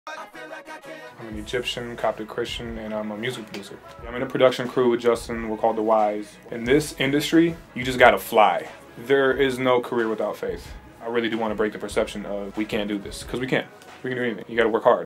Arab Stories - Rob Knox Teaser
Here is a teaser for the next episode of our Arab Stories documentary series featuring music producer Rob Knox who worked with Rihanna, Chris Brown, Britney Spears, Leona Lewis etc. and who currently is in a production company with Justin Timberlake.